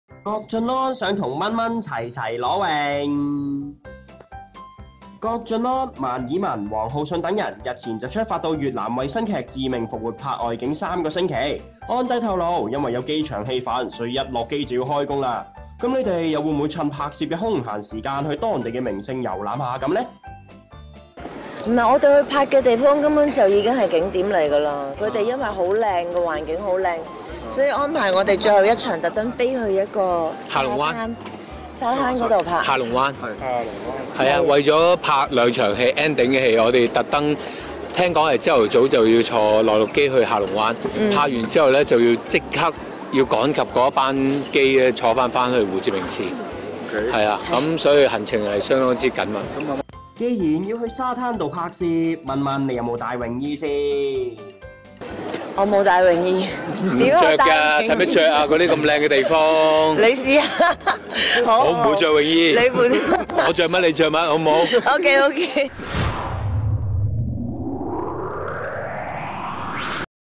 郭晉安想同蚊蚊齊齊裸泳
郭晉安(安仔)、萬綺雯(蚊蚊)、王浩信等人，清晨出發到越南為新劇《致命復活》拍外景三星期。安仔透露因為有機場戲份，所以一落機就要開工。問到會否趁空閒時間到當地的名勝遊覽，蚊蚊表示：「我哋去拍攝嘅地方就係名勝嚟，因為嗰度啲環境好靚，所以安排咗我哋最後一場去沙灘拍，去下龍灣拍。拍完就要即刻趕機返去胡志明市，行程相當緊密！」而問到蚊蚊可有帶備比堅尼泳衣，她笑言：「我冇帶泳衣，我淨係着戲服。」